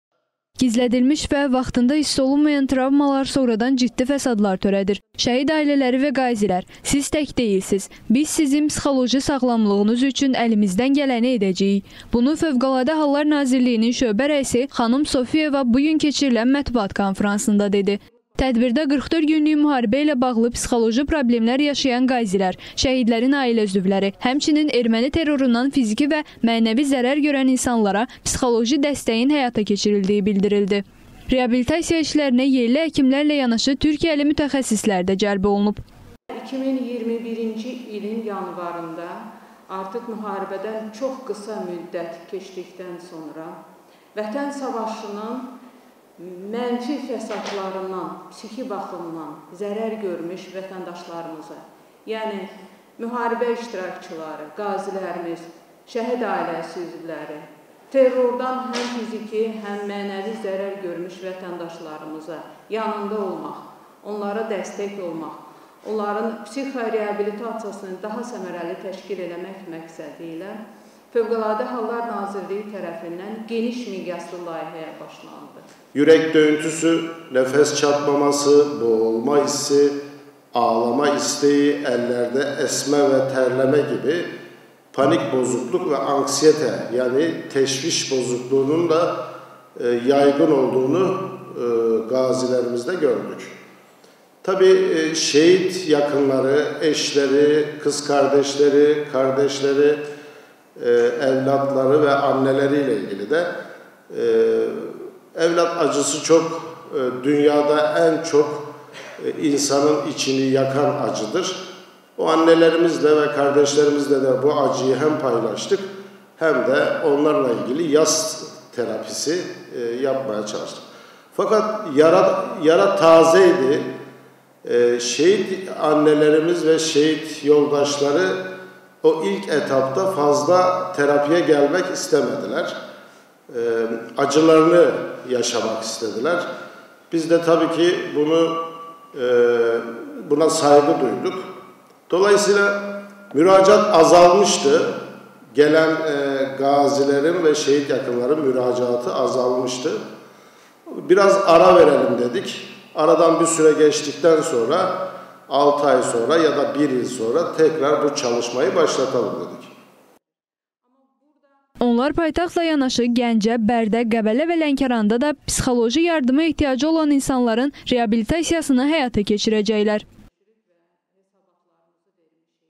FHN psixoloji yardımlara davam edir
Türkiyəli mütəxəssislər də reabilitasiya işlərinə cəlb olunacaqlar